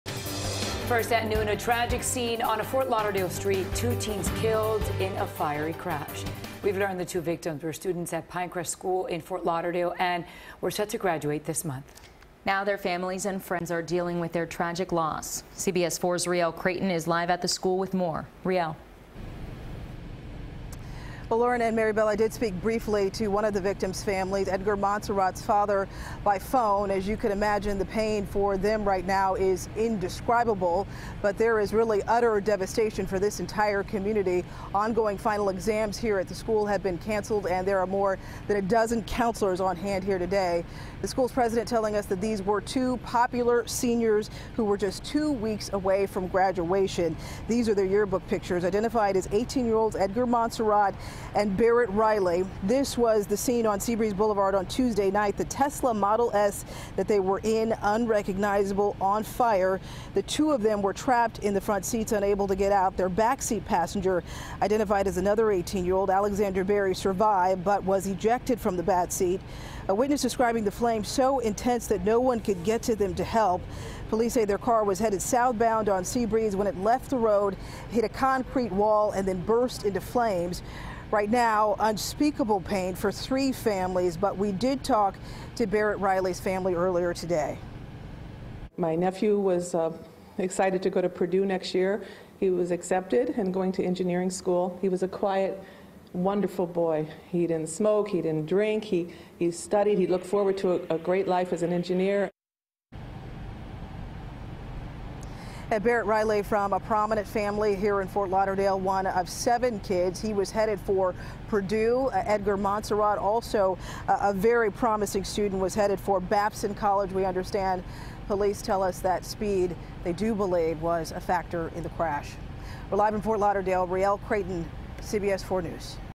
High School Seniors Killed In Fiery Ft. Lauderdale Crash
Rielle Creighton reports Barrett Riley and Edgar Monserratt were just two weeks away from graduation.